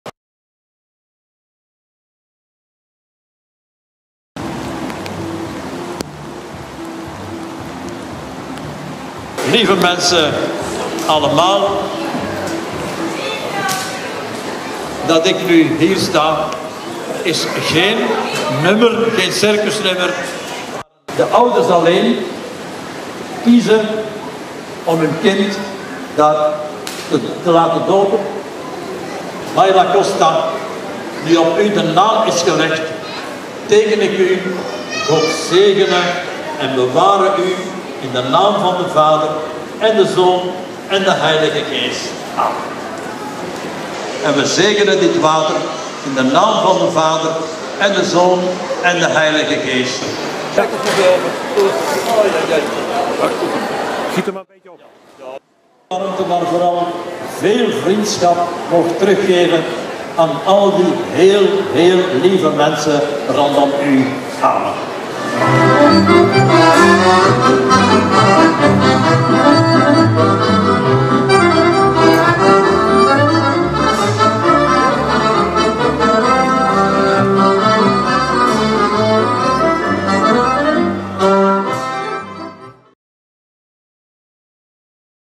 Doop Mayla-Costa in piste Circus Renaissance
Een unieke gebeurtenis zondag in de goedgevulde circustent van circus Renaissance op Het Laar in Tilburg. Bij aanvang van de voorstelling  werd Mayla-Costa gedoopt midden in de circuspiste . Mayla-Costa, zij is de dochter van Mike-Wesley Benneweis en Corien Lutgers en geboren op 25 juli. De vader van de kleine is iemand die zijn roots in de circuswereld heeft want hij stamt uit het Deense circus Benneweis en het vroegere Nederlandse circus Mullens.
De vierentachtigjarige circus-pastoor Kristiaan van Der Linde uit België leidde de doopdienst.. Direct na de doopplechtigheid begon het circus aan zijn middagvoorstelling. Vroeger was het een traditie dat kinderen van circusartiesten in de piste werden gedoopt. Maar het is inmiddels jaren geleden dat dit nog gebeurd is. Het Hooggeëerd publiek heeft volop genoten van de unieke gebeurtenis.